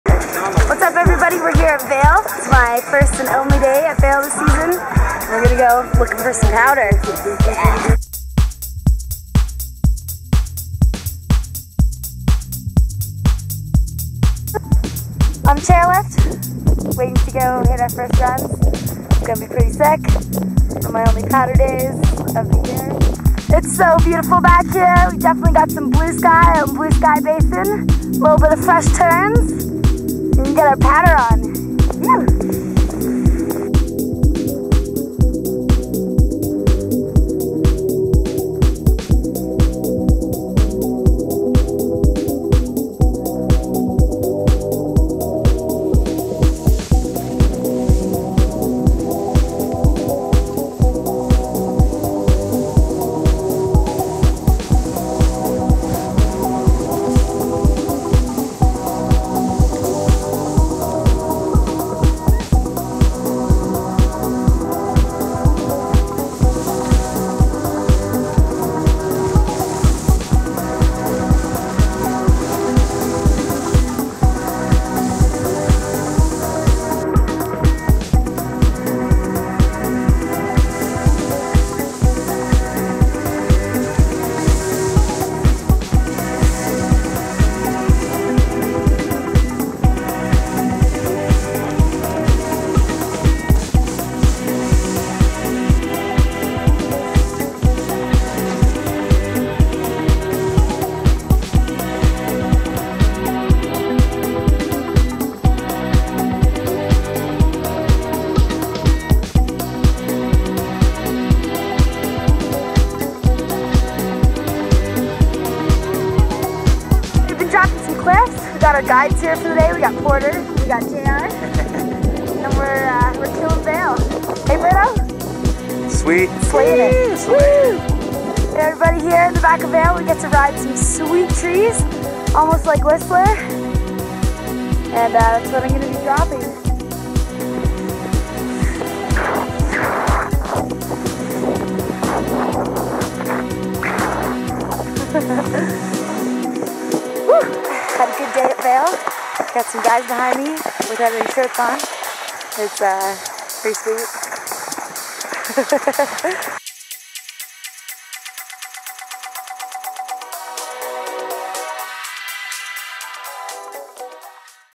Katie Tsuyuki's Video Blog 30: Powder Riding @ Vail
Take powder turns with Katie Tsuyuki at Vail Resort, CO. ;Filmed with Sony Handycam HDR XR150 and Cyber-shot DSC HX5